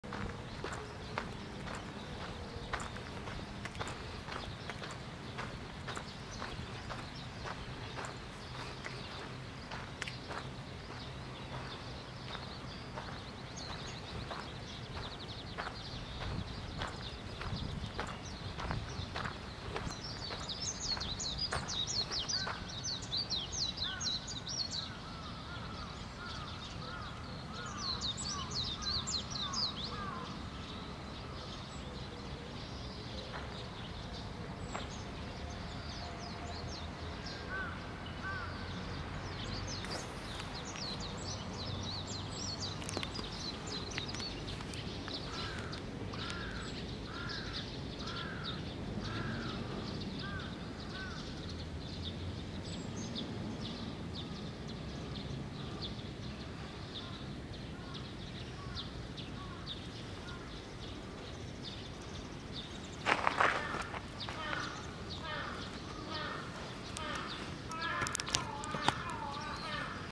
サリーのドッグラン
早朝の公園で、ドッグラン。全然走らないサリー。